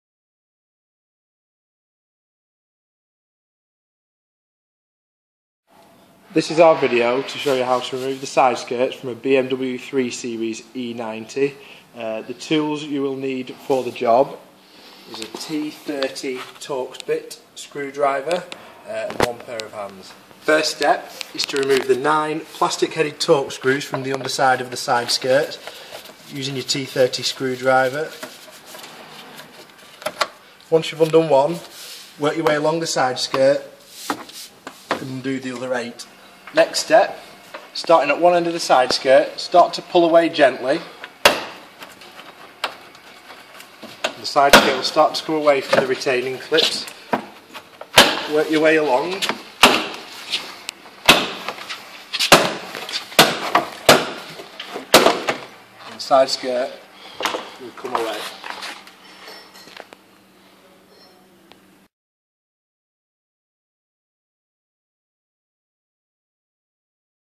BMW 3-SERIES E90 / E91 / E92 / E93 2005-2012 - How To Remove Side Skirt / Rocker Panel Removal
Guide on how to remove the side skirts from a BMW 3-Series (E90 / E91 / E92 / E93) years 2005 to 2012.

Tools used:

-T30 Torx (included in this set, good for most small jobs)
-Silverline Trim Tools (5-piece set) (11-piece set)